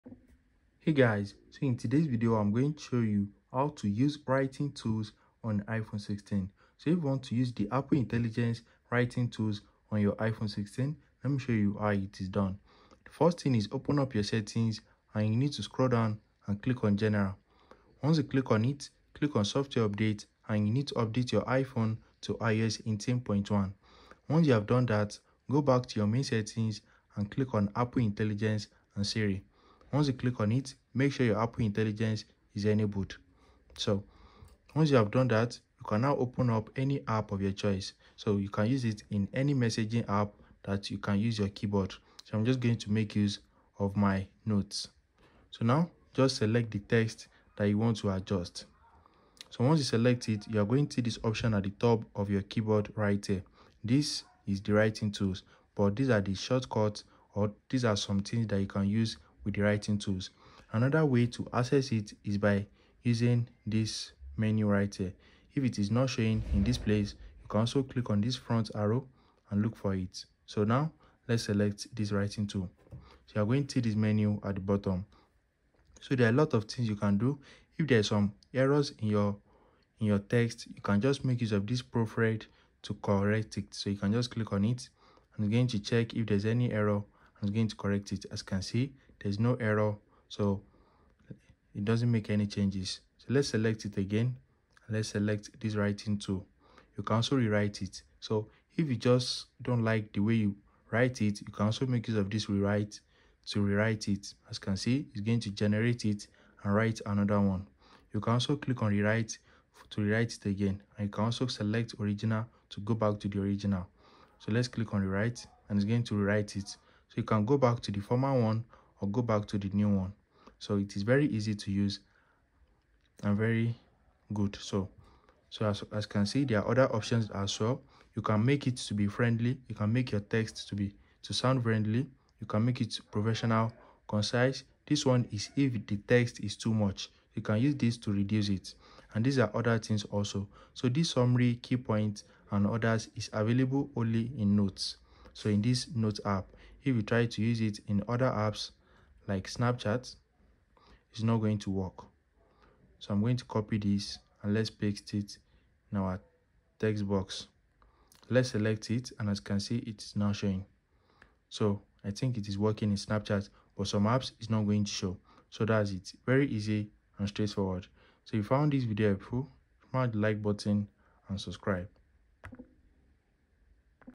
How to Use Writing Tools on iPhone 16
Want to elevate your writing on your iPhone 16? Learn how to use powerful writing tools like proofread, rewrite, and Concise text to enhance your productivity and creativity. This video will guide you through the features and tips to improve your writing experience.